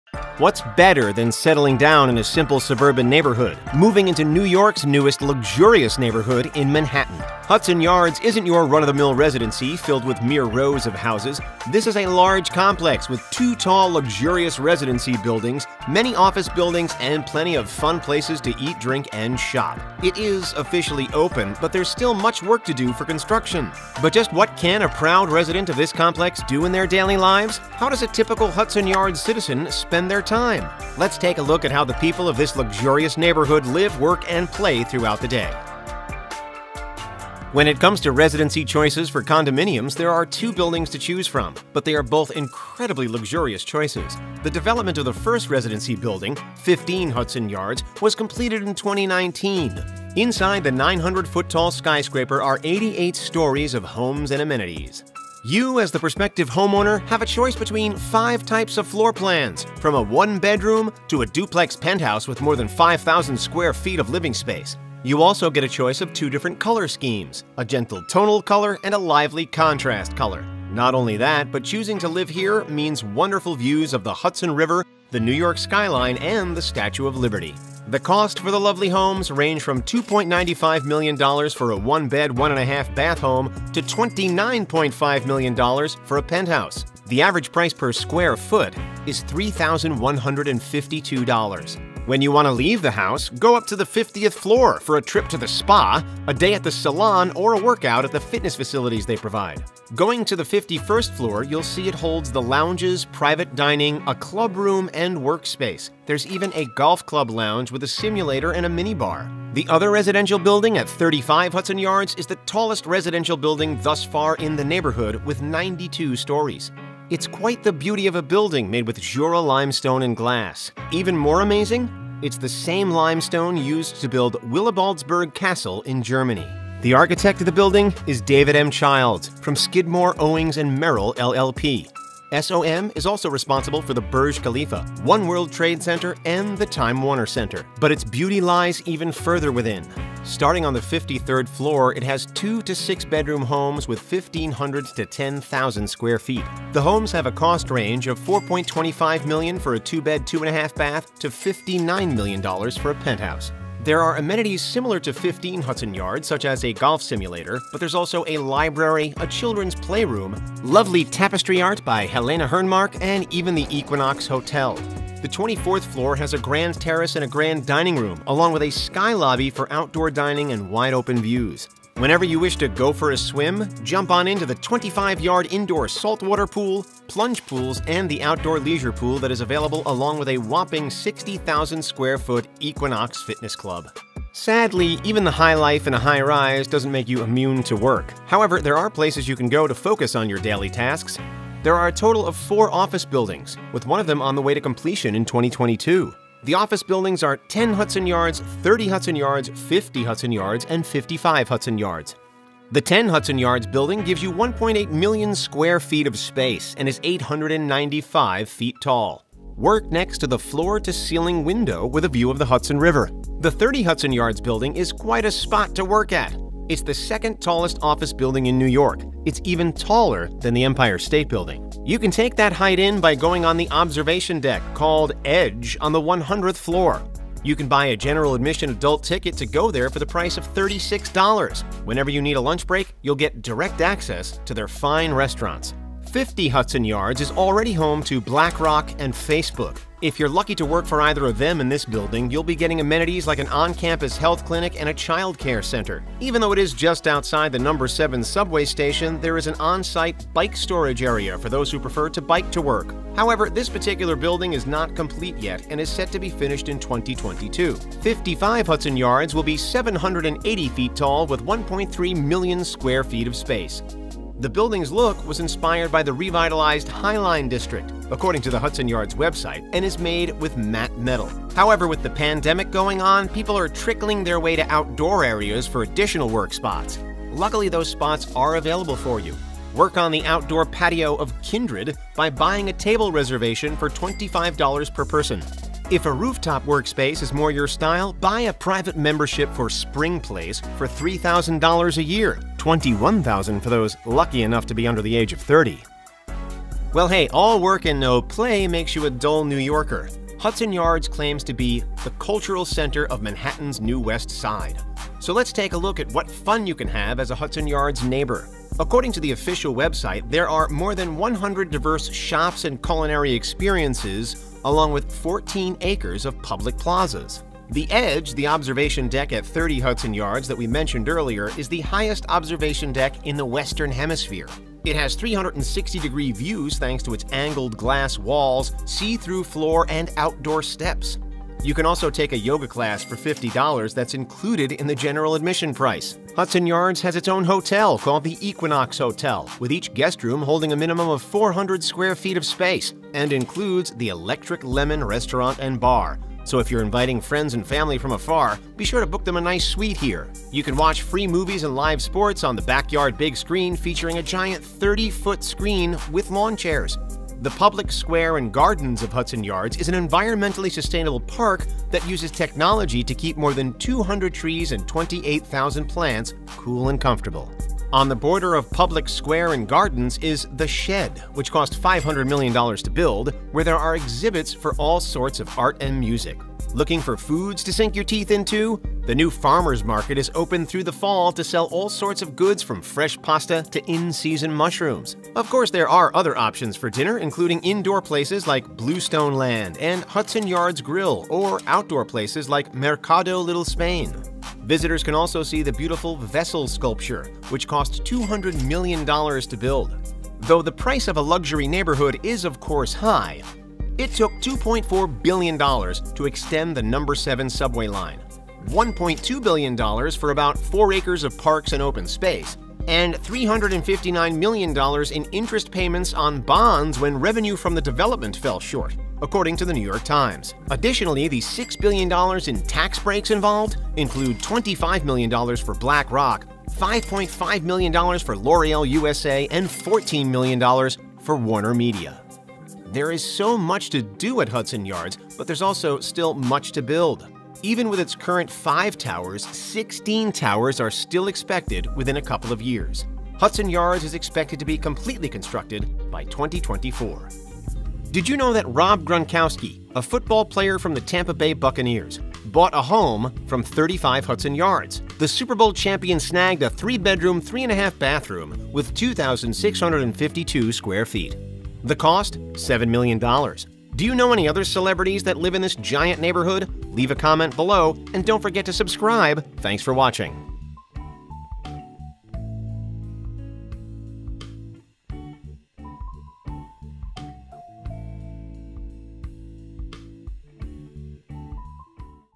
New York's New Private Community For The Super Rich
Where do you go when you want to move to somewhere new? New York’s Hudson Yards is the answer. Not only would it be a new location, but it is also a brand new $25 billion dollar neighborhood still under construction. You get to choose between two luxurious residency towers to live in and many different places to work and relax. The cost for this neighborhood is massive, but from seeing the amenities—from the many pools to swim in, to the fantastic views of some of the New York landmarks, to the fabulous places to work at. You can also visit fun places like the culture-filled Shed, the Vessel sculpture, the highest observation tower in the Western Hemisphere called the Edge, and even eat from the Farmers’ Market. But just what is inside each residency? What is special about each office building? What can you do for fun at the Hudson Yards? In this video, we will talk about the ins and outs of the skyscrapers that are currently up and benefits of being a resident of this large neighborhood including where you can work whether you prefer working indoors or outdoors. We will also talk about what money went in to build this neighborhood up. Interested in moving here? Then if you want to grab a home at the Hudson Yards, simply go to their to learn more about the neighborhood. But before you send that email or make that phone call, watch our video to know what to expect as a resident! And be sure to watch to the end of the video to find out what sports celebrity has a home for himself in 35 Hudson Yards right now!

Written by: Hannah Victoria Johnson
Narrated by: Peter DeGiglio
Edited by: Andrew Gut

CHAPTERS:
00:00 Intro
00:38 Living in Hudson Yards
03:04 Working in Hudson Yards
04:57 Playing in Hudson Yards